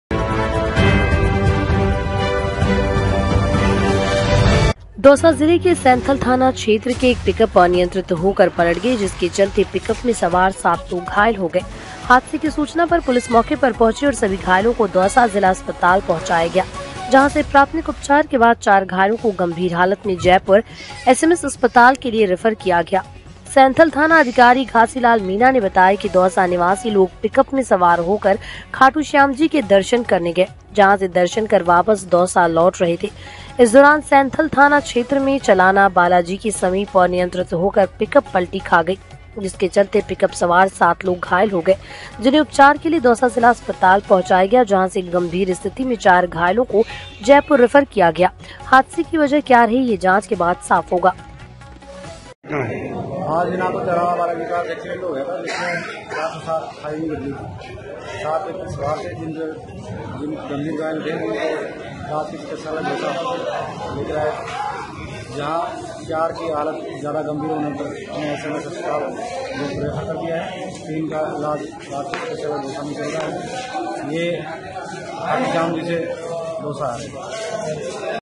दौसा जिले के सैंथल थाना क्षेत्र में एक पिकअप अनियंत्रित होकर पलट गई
A pickup overturned uncontrolled in the Santhal police station area of ​​Dausa district.